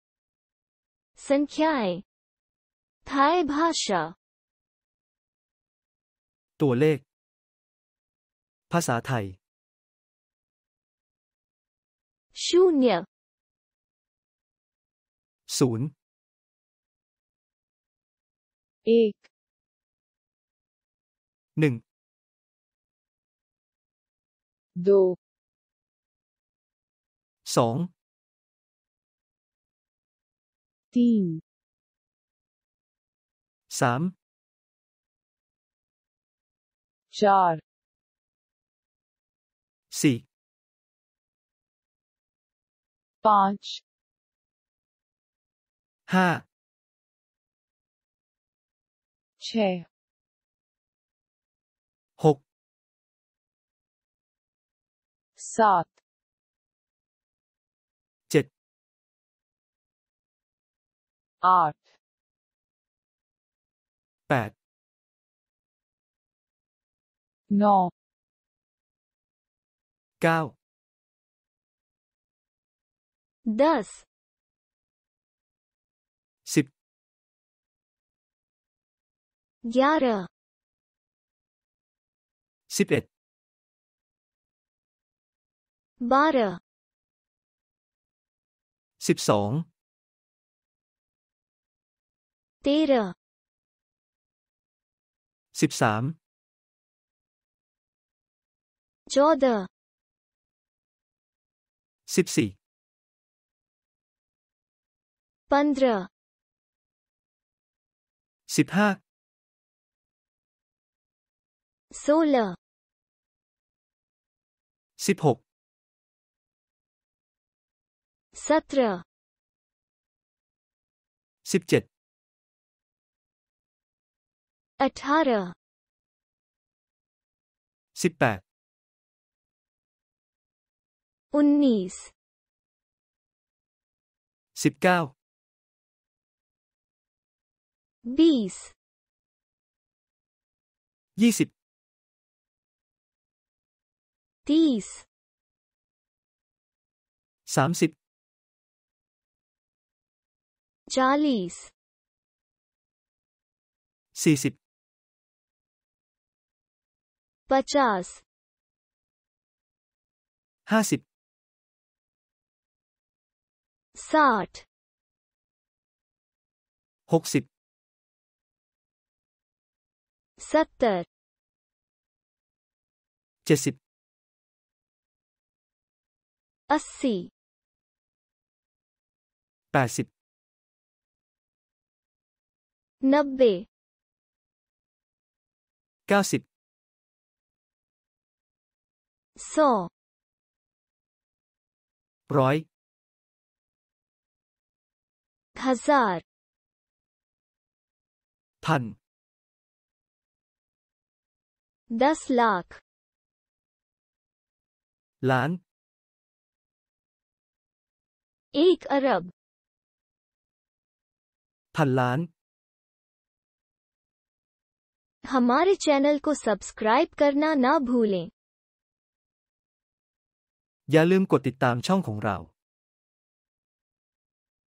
संख्याएँ (थाई भाषा) (hi-th)
Fone TV
hi-th-4
ตัวเลข (ภาษาไทย)
शून्य - ศูนย์
एक - หนึ่ง
दो - สอง
तीन - สาม
चार - สี่
पांच - ห้า
छह - หก
सात - เจ็ด
आठ - แปด
नौ - เก้า
दस - สิบ
ग्यारह - สิบเอ็ด
बारह - สิบสอง
तेरह - สิบสาม
चौदह - สิบสี่
पंद्रह - สิบห้า
सोलह - สิบหก
सत्रह - สิบเจ็ด
अठारह - สิบแปด
उन्नीस - สิบเก้า
बीस - ยี่สิบ
तीस - สามสิบ
चालीस - สี่สิบ
पचास - ห้าสิบ
साठ - หกสิบ
सत्तर - เจ็ดสิบ
अस्सी - แปดสิบ
नब्बे - เก้าสิบ
सौ - ร้อย
हज़ार - พัน
दस लाख - ล้าน
एक अरब - พันล้าน